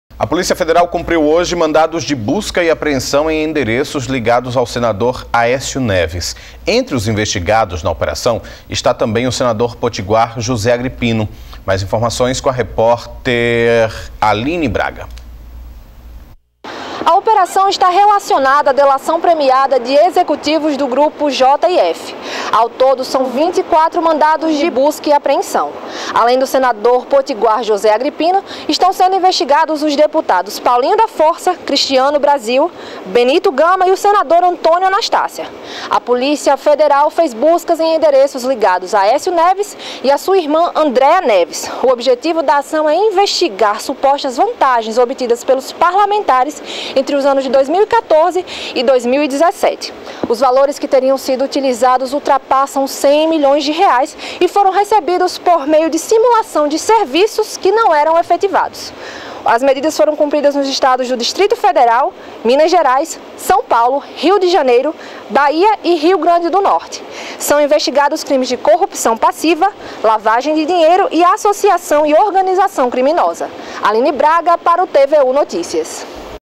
Senador José Agripino é investigado pela Polícia Federal na Operação Ross.
Repórter: Aline Braga
Imagens: Leonardo Figueiredo